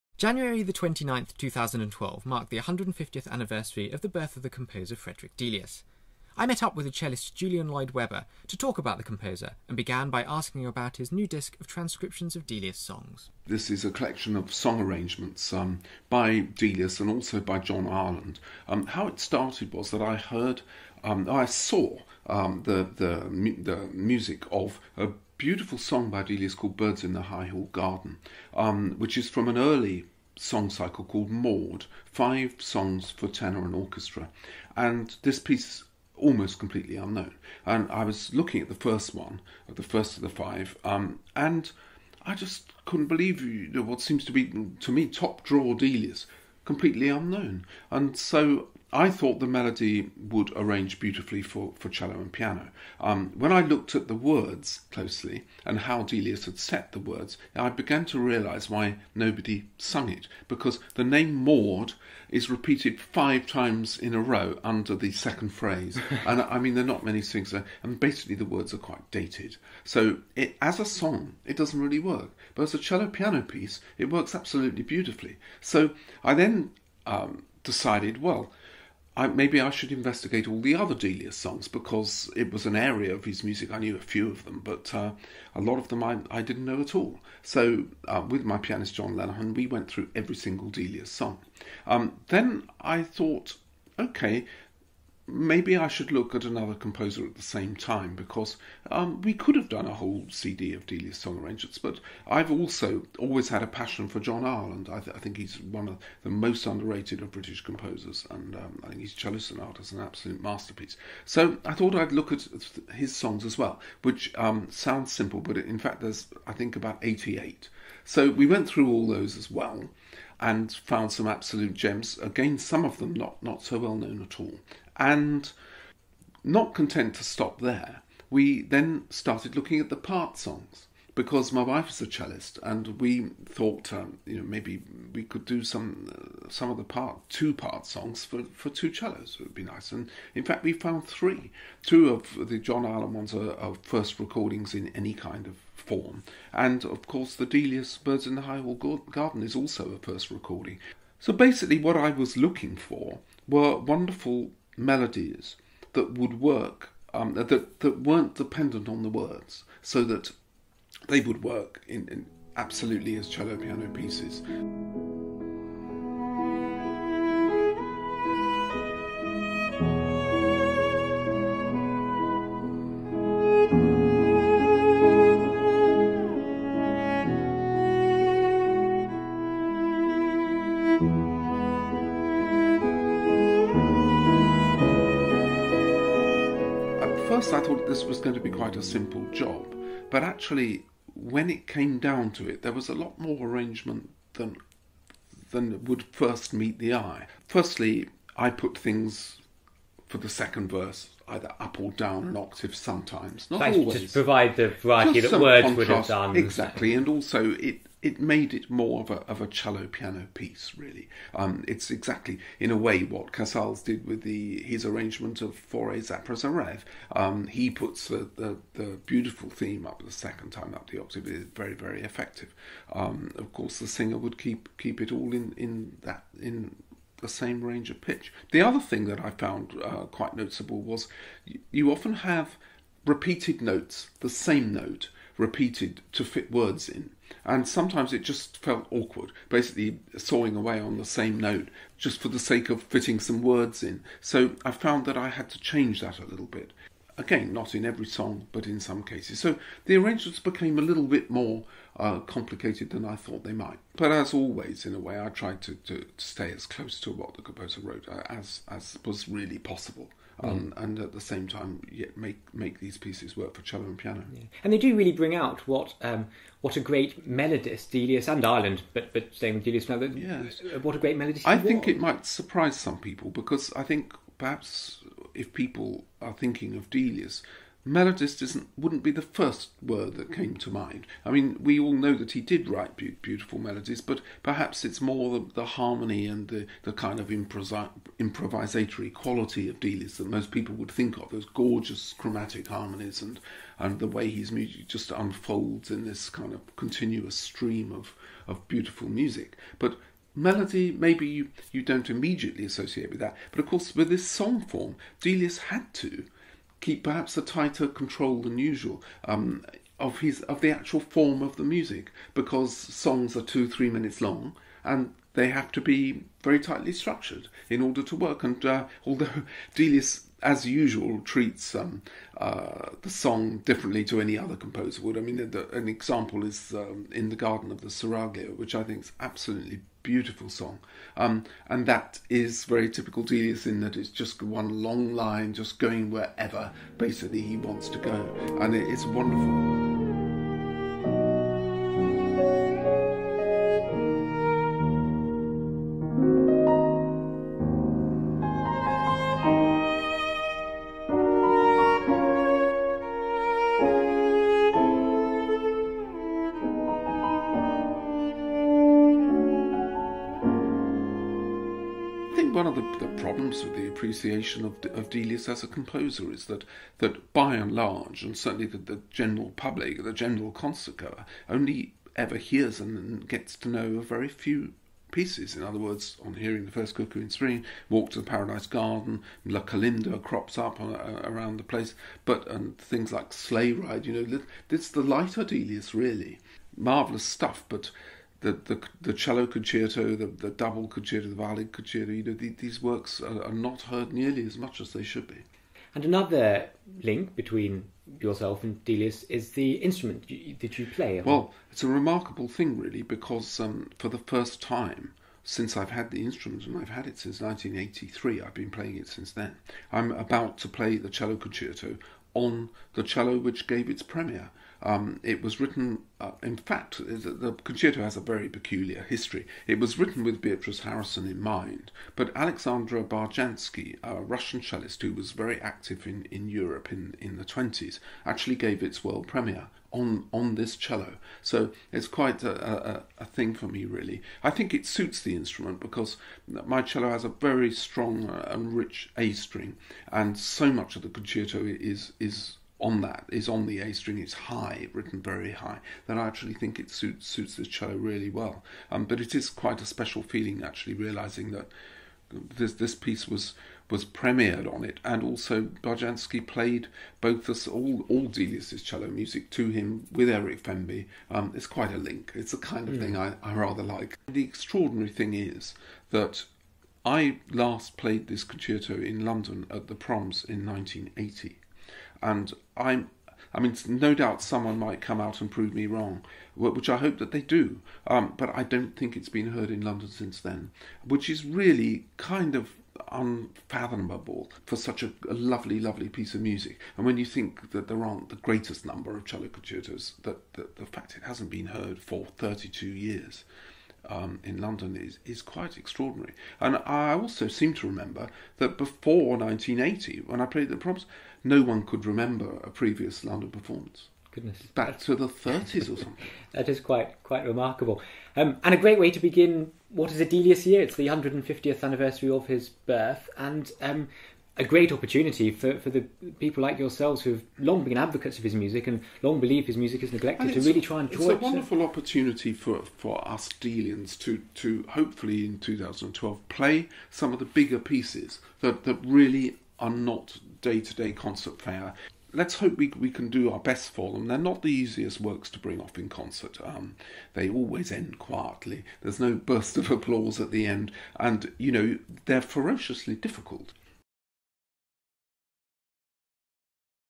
Julian Lloyd Webber on Delius - Gramophone Interview
2012 marks 150 years since the birth of the composer Frederick Delius.
Gramophone Editor, Martin Cullingford, spoke to the cellist Julian Lloyd Webber about his recent disc of songs by Frederick Delius and John Ireland, arranged for cello and piano.

Photo Credit: Simon Fowler